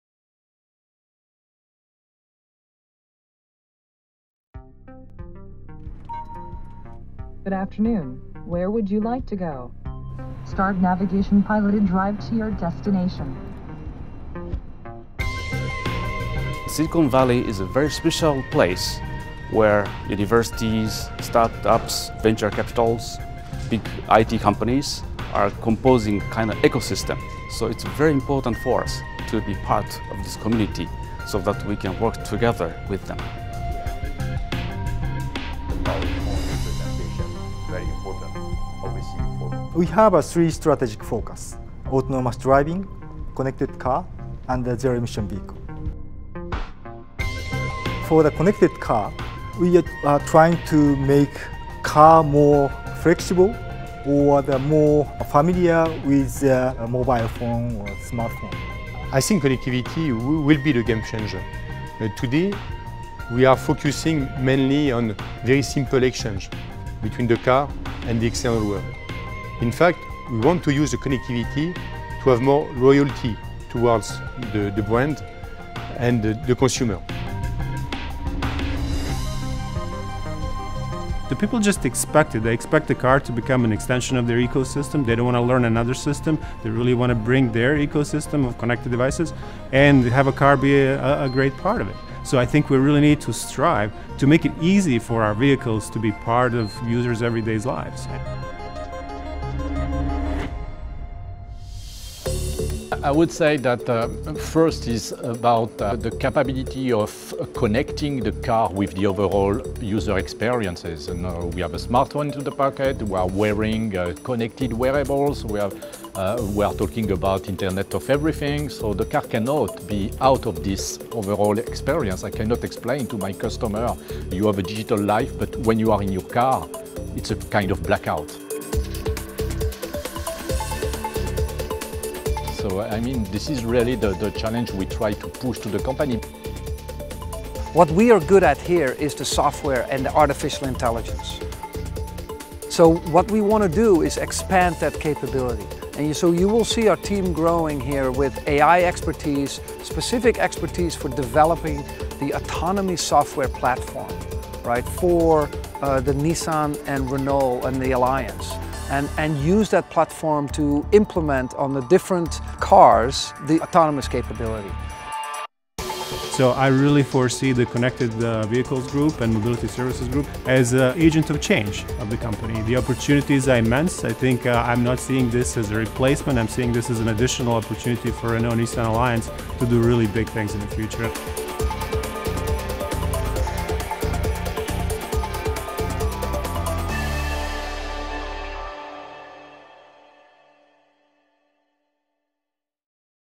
法國雷諾汽車與日本日產汽車合作發展科技
2016年法國雷諾 - 日本日產聯盟在矽谷研究中心.在科技創新.學術研究.創業投資.以及政府和航空航天...等等項目聯盟共同發展.且讓我們拭目以待.